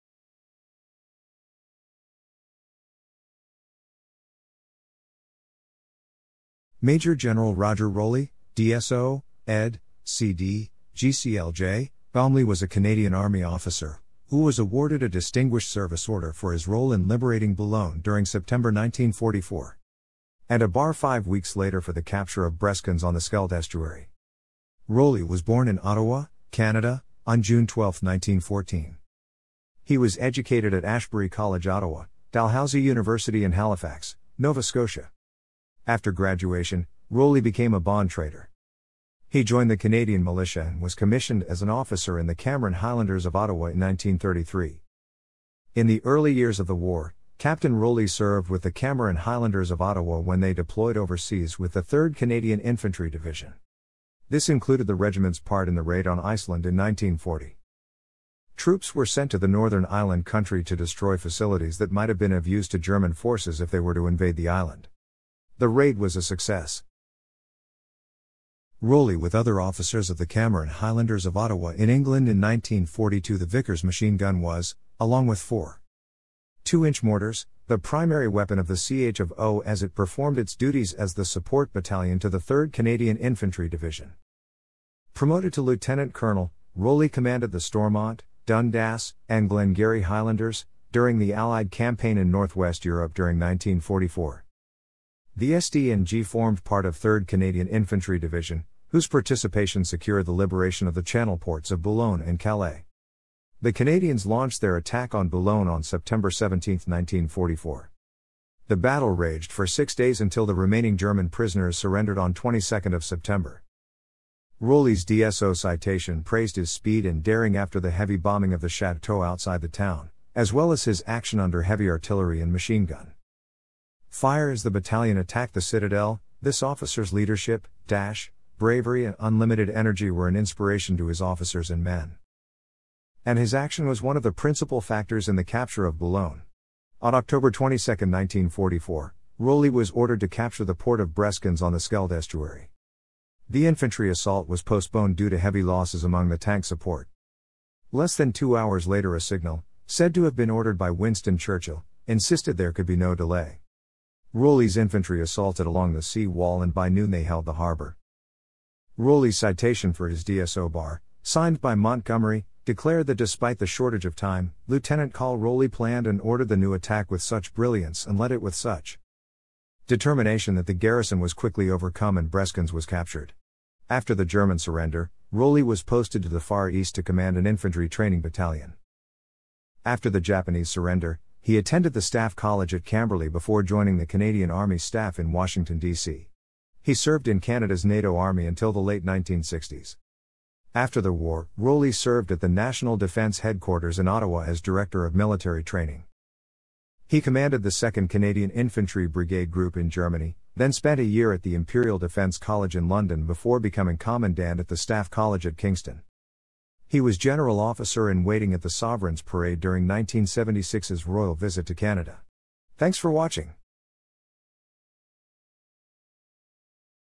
Roger Rowley
Major General Roger Rowley, DSO, ED, CD, GCLJ, GOMLJ （12 June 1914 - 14 February 2007） was a Canadian Army officer who was awarded a Distinguished Service Order for his role in liberating Boulogne during September 1944,
and a Bar five weeks later for the capture of Breskens on the Scheldt estuary.
Rowley was born in Ottawa, Canada, on 12 June 1914.
He was educated at Ashbury College Ottawa, Dalhousie University in Halifax, Nova Scotia.
After graduation, Rowley became a bond trader.
He joined the Canadian militia and was commissioned as an officer in the Cameron Highlanders of Ottawa in 1933.
In the early years of the war, Captain （ and later Major） Rowley served with the Cameron Highlanders of Ottawa when they deployed overseas with the 3rd Canadian Infantry Division.
This included the regiment's part in the raid on Iceland in 1940.
Troops were sent to the northern island country to destroy facilities that might have been of use to German forces if they were to invade the island.
The raid was a success.
Rowley （rear, right） with other officers of the Cameron Highlanders of Ottawa in England in 1942 The Vickers machine gun was, along with 4.
2-inch （110 mm） mortars, the primary weapon of the CH of O as it performed its duties as the Support Battalion to the 3rd Canadian Infantry Division.
Promoted to Lieutenant Colonel, Rowley commanded the Stormont, Dundas and Glengarry Highlanders, during the Allied campaign in North West Europe during 1944.
The SD&G formed part of 3rd Canadian Infantry Division, whose participation secured the liberation of the Channel ports of Boulogne and Calais.
The Canadians launched their attack on Boulogne on 17 September 1944.
The battle raged for six days until the remaining German prisoners surrendered on 22 ...